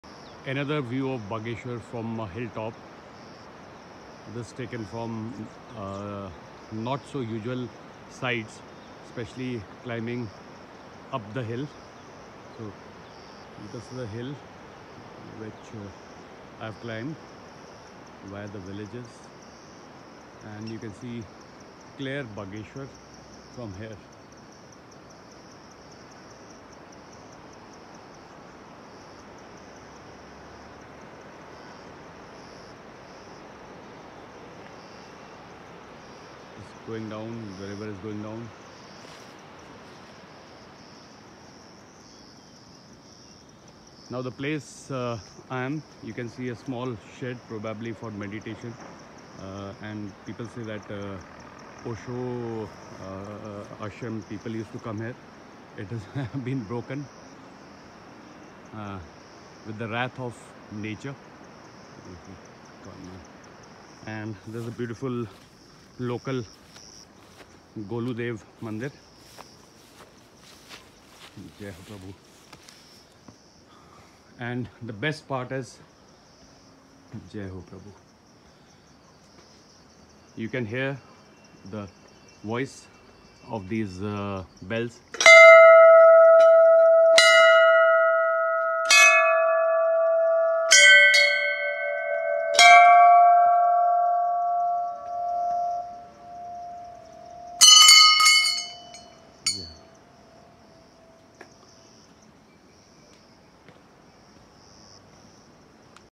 View of Bageshwar town from hill top
You would be happy hearing temple bells